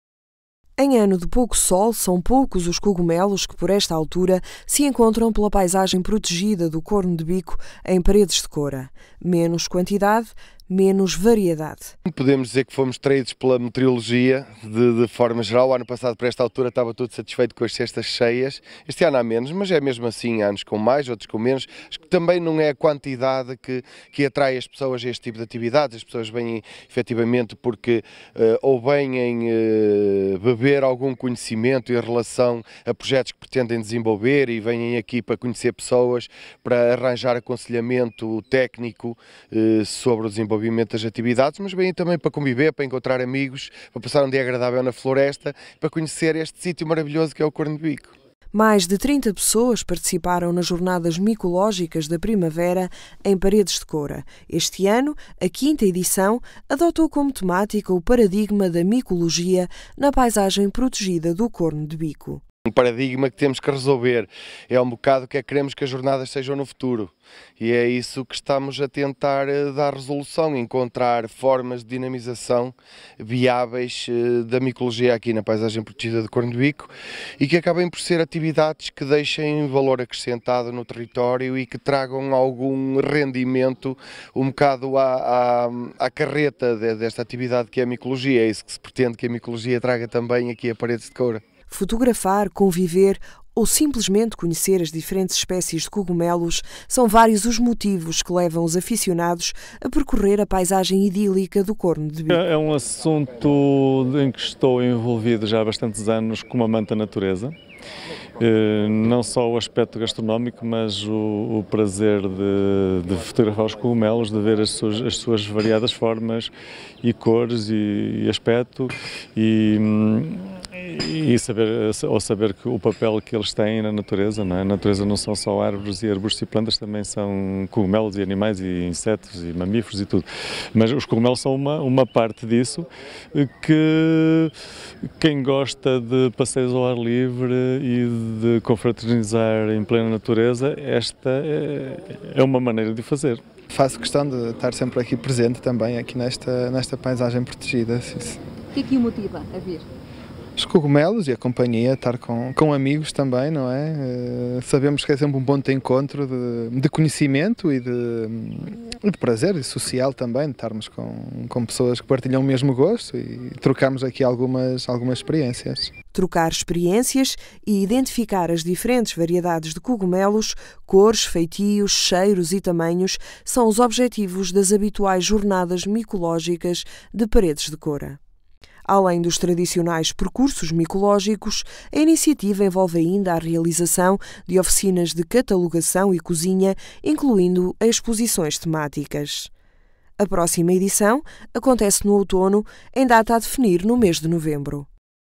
Corno de Bico acolhe Jornadas Micológicas da Primavera | Altominho TV
Mais de 30 pessoas participaram nas Jornadas Micológicas da Primavera, em Paredes de Coura. Em ano de pouco sol, são poucos os cogumelos que, por esta altura, se encontram pela Paisagem Protegida do Corno de Bico.